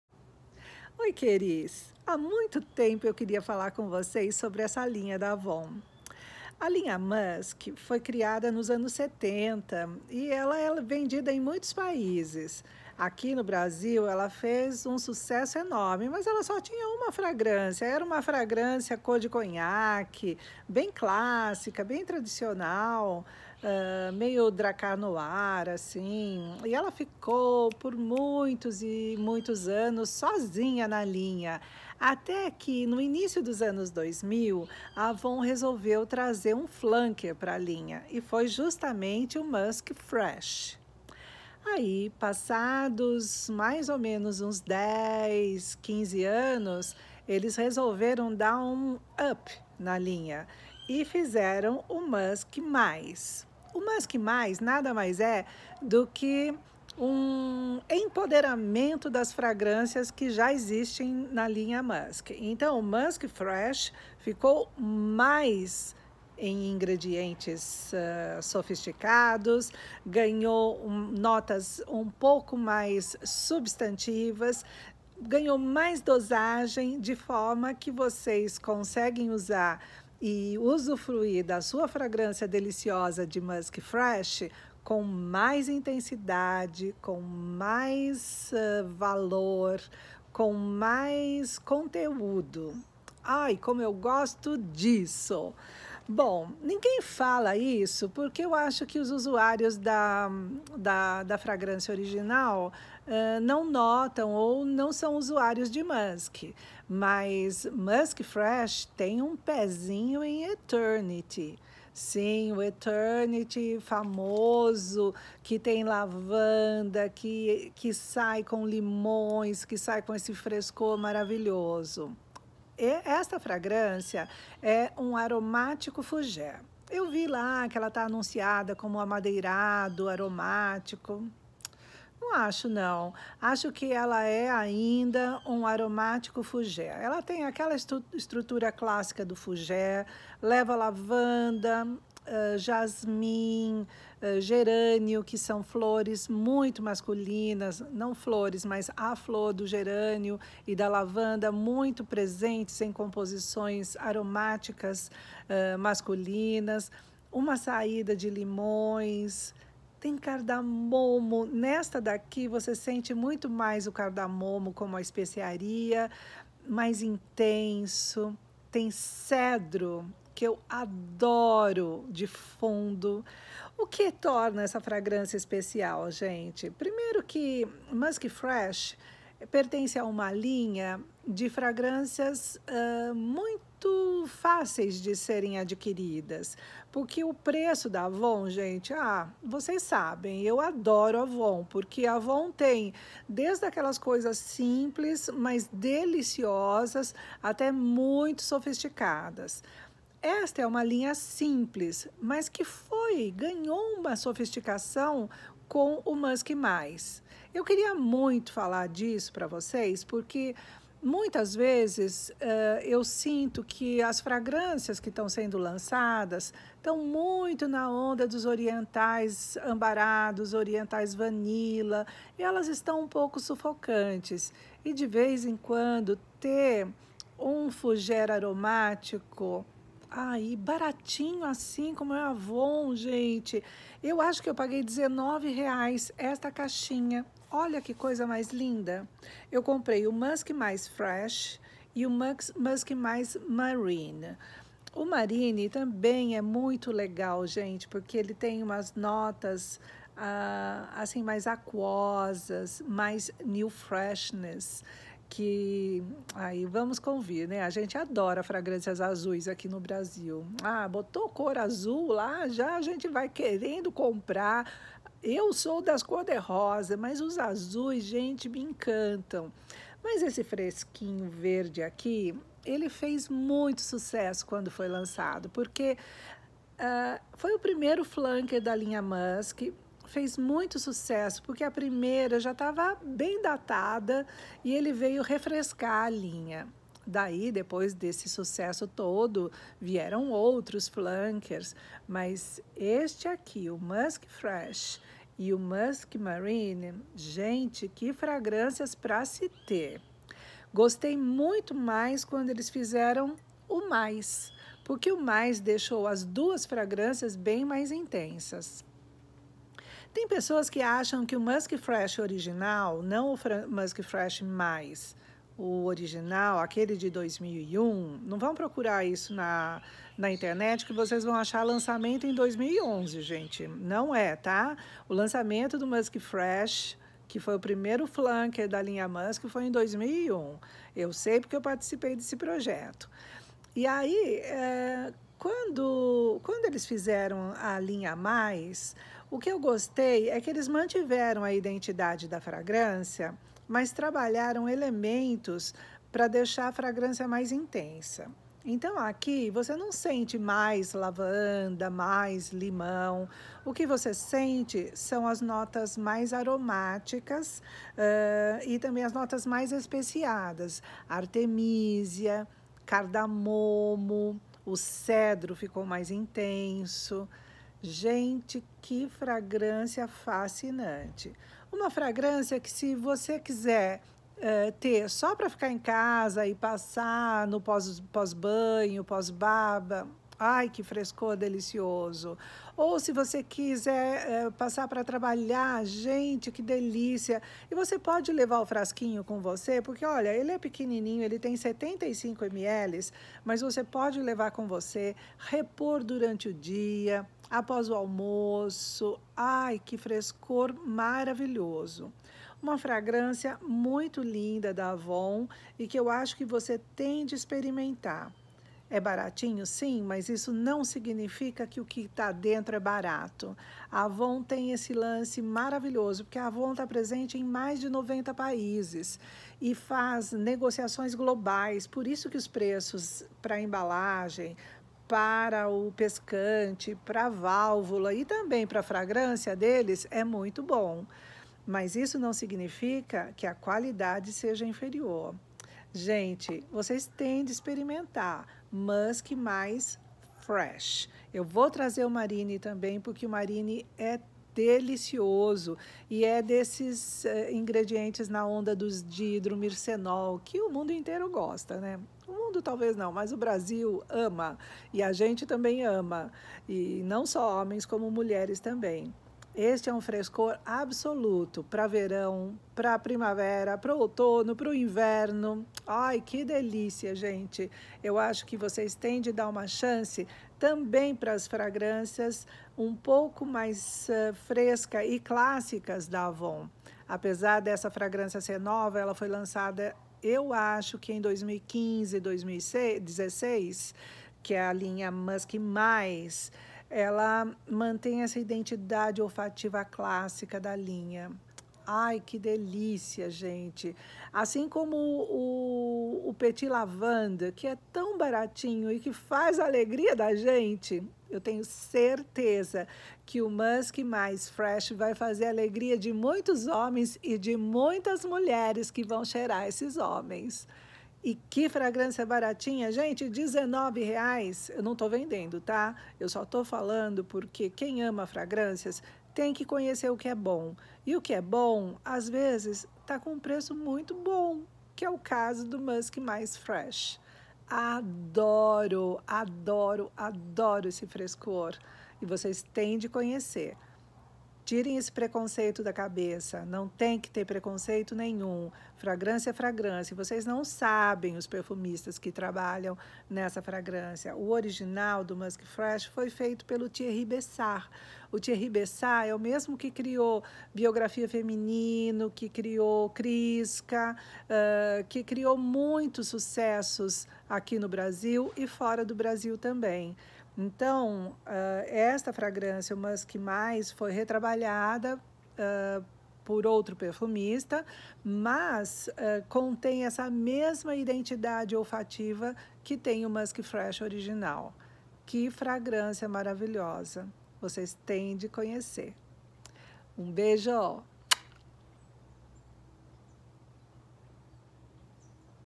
Musk (+) Fresh, da Avon, um frescor pra encantar
A linha Musk foi criada pela Avon perto dos anos 80 e se manteve com uma fragrância por muito tempo, até que resolveu criar seu primeiro Flanker: Musk Fresh - um sucesso, desde seu lançamento. Passados cerca de 15 anos, a Avon cria Musk (+) para tornar as fragrâncias da linha Musk ainda mais substantivas e sofisticadas. Musk (+) Fresh é um Fougere  Aromático que ganhou mais intensidade nas madeiras e notas especiadas, mas que mantém a identidade e o frescor do original Musk Fresh. Uma linha deliciosa e casual, pra ser usada em diversas ocasiões ao ar livre, no trabalho... Uma deo colônia com precinho de splash. Dessas coisas que só a Avon faz por nós.